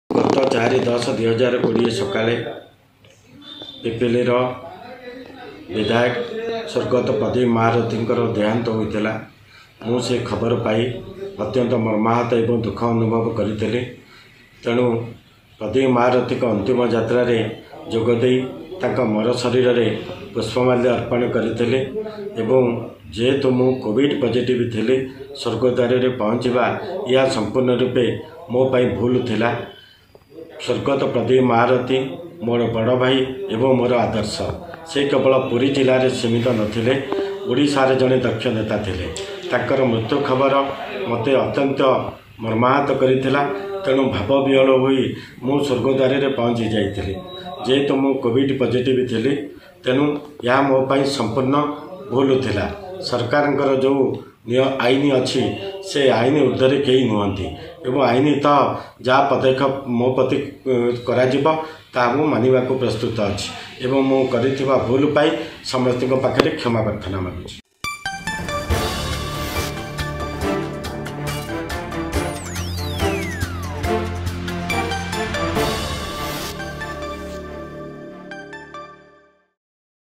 କ୍ଷମା ପ୍ରାର୍ଥନା କଲେ ସତ୍ୟବାଦୀ ବିଧାୟକ ଉମା ସାମନ୍ତରାୟ.....
କୋଭିଡ ନିୟମ ଉଲଂଘନ କରି ଥିବାରୁ କ୍ଷମା ପ୍ରାର୍ଥନା କଲେ ସତ୍ୟବାଦୀ ବିଧାୟକ ଉମା ସାମନ୍ତରାୟ.